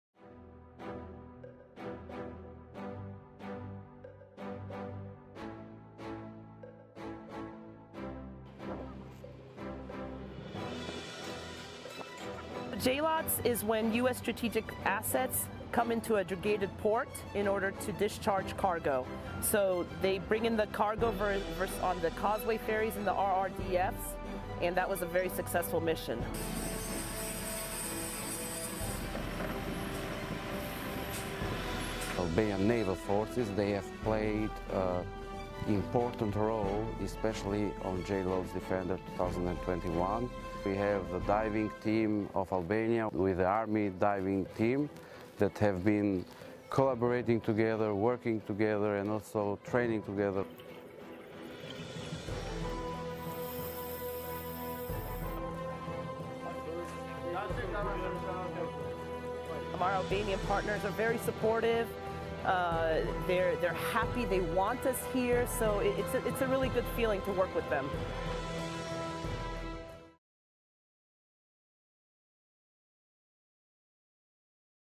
Joint Logisitics Over-the-Shore DEFENDER-Europe 21
DURRES, ALBANIA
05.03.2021

Joint Logistics Over-the-Shore also known as JLOTS, took part of the theater-opening exercise Immediate Response linked to DEFENDER-Europe 21 on May 3, 2021, in Durres, Albania. JLOTS is a unified commander’s joint employment of Army and Navy LOTS assets to deploy and sustain combat forces. JLOTS operations allow US strategic sealift ships to discharge and download equipment and personnel in austere environments, damaged ports, or over a bare beach.. DEFENDER-Europe is an annual large-scale U.S. Army-led, multinational, joint exercise designed to build readiness and interoperability between U.S., NATO and partner militaries. This year, more than 28,000 multinational forces from 27 nations will conduct nearly simultaneous operations across more than 30 training areas in more than a dozen countries from the Baltics to the strategically important Balkans and Black Sea Region. Follow the latest news and information about DEFENDER-Europe 21,

Film Credits: Video by Spc. Daria Jackson

EUCOM
USArmy
StrongerTogether
USAREURAF
DefenderEurope